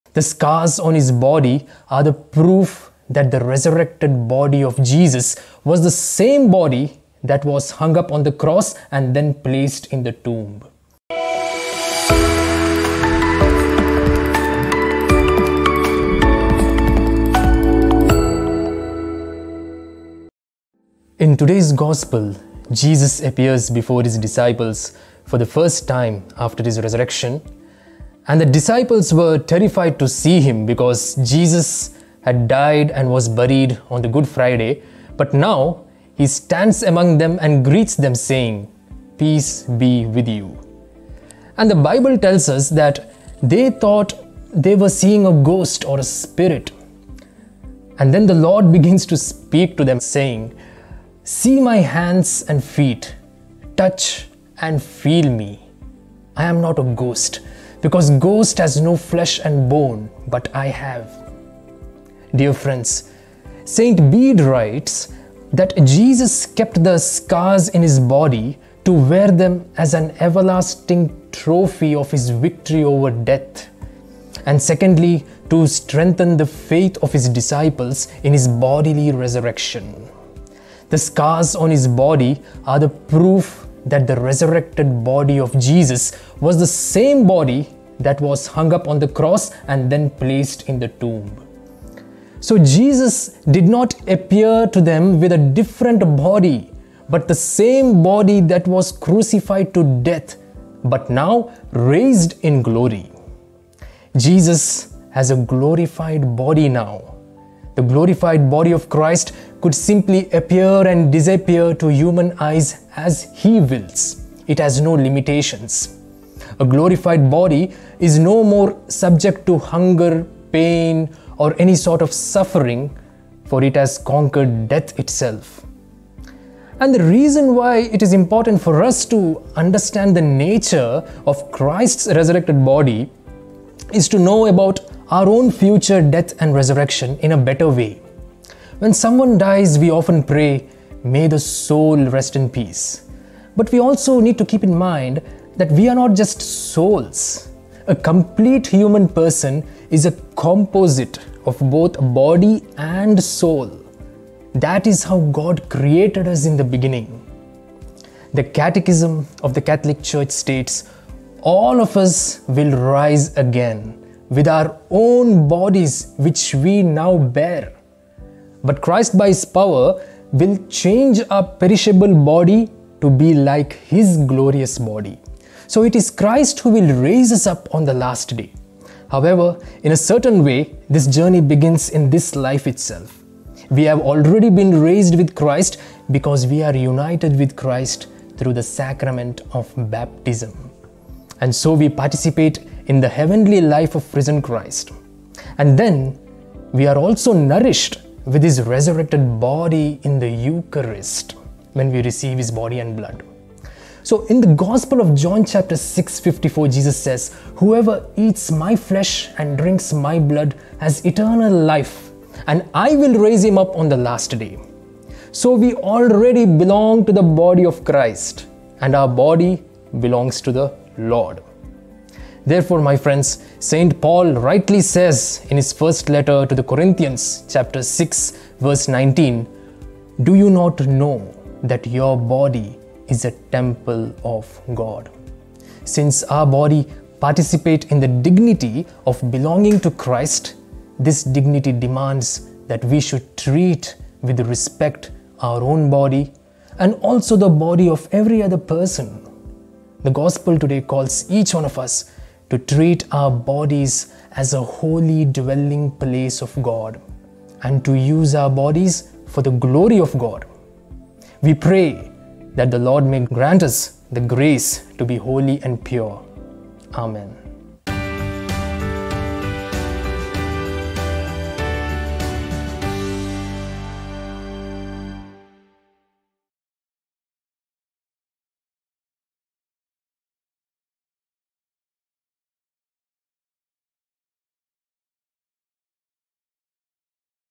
13/04/23 | Reflection 1770 | Br Malcolm Concessao OP | Luke 24:35-48
In today’s Gospel, Jesus appears before his disciples for the first time after his resurrection, And the disciples were terrified to see him because Jesus had died and was buried on the Good Friday but now, he stands among them and greets them saying “peace be with you”. And the bible tells us that they taught they were seeing a ghost. And the Lord begins to speak to them saying, “see my hands and feet, touch and feel me. I am not a ghost, because ghost has no flesh and body, but I have one.
Dear friends, St Bede writes, That Jesus kept the scars in his body to wear them as an everlasting trophy of his victory over death and secondly to strengthen the faith of his disciples in his bodily resurrection. The scars on his body are the proof that the resurrected body of Jesus was the same body that was hung up on the cross and then placed in the tomb. So, Jesus did not appear with a different body, But the same body that was crucified to death, but now raised in Glory.
Jesus has a glorified body now, the glorified body of Christ could simply appear and disappear to human eyes as he wills, it has no limitations. A glorified body is no more subject to hunger, pain or any sort of suffering for it has conquered death itself and the reason why it is important to understand the nature of Christ’s resurrected Body, is to know about our own future death and resurrection in a better way. When someone dies, we all pray, May the soul rest in peace. But we are not just souls, A complete human person is a composite of both body and soul, that is how God created us in the beginning.
The catechism of the catholic church states, "all of us will rise again with our own bodies which we now bear," but Christ by his power "will change our perishable body to be like his glorious body” So it is Christ who will raise us up on the last day. However, in a certain way, this journey begins in this life itself, we have already been raised with Christ, because we are United with Christ through the sacrament of Baptism and so we participate in the heavenly life of the risen Christ and then we are also nourished with his resurrected body in the Eucharist. In the Gospel of John chapter 6 verse 54 Jesus says, “whoever eats my flesh and drinks my blood, has eternal life, and I will raise him up on the last day” so we already belong to the Body of Christ and our Body belongs to the Lord.
Therefore St. Paul rightly says in his first letter to Cor chapter 6 verse 19, “Do you not know that your body is a temple of God”. Since our Body participate in the dignity of belonging to Christ. This dignity demand’s that we should treat with respect our own body, and also the body of every other person. The Gospel Today calls each one of us to treat our bodies as a holy dwelling place of God and to use our bodies for the Glory of God. We pray that the Lord may grant us the Grace to be Holy and pure. Amen.